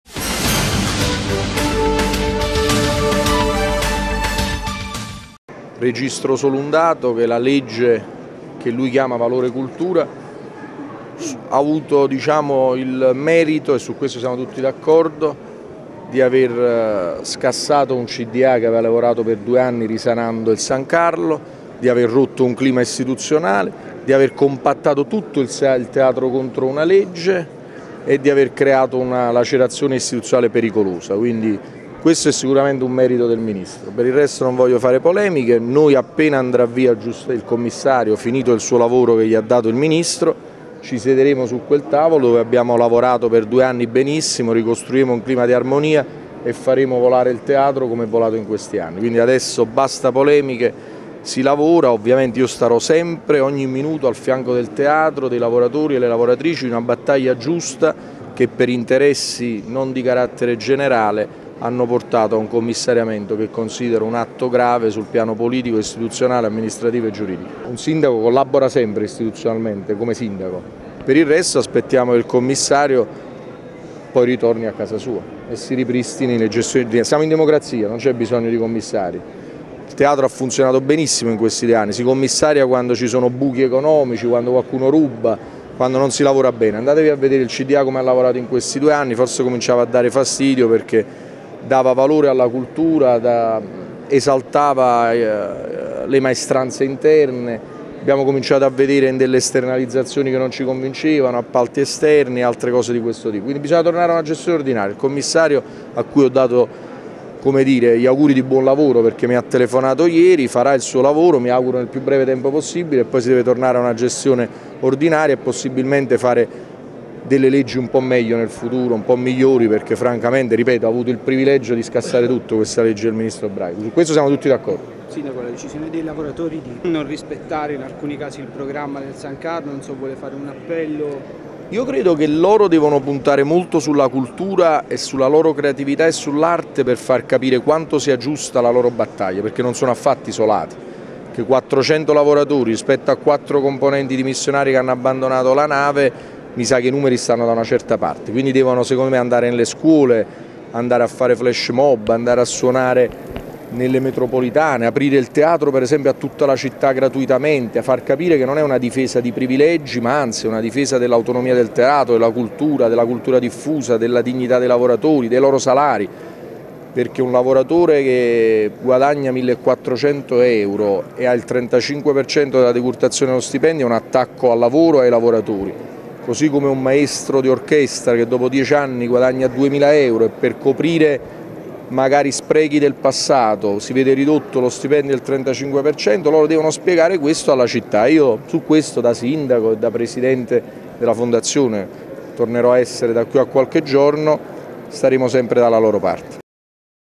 De Magistris: sempre al fianco dei lavoratori del San Carlo
Il sindaco Luigi de Magistris commenta il commissariamento del Teatro San Carlo a margine dell'inaugurazione dell'anno giudiziario.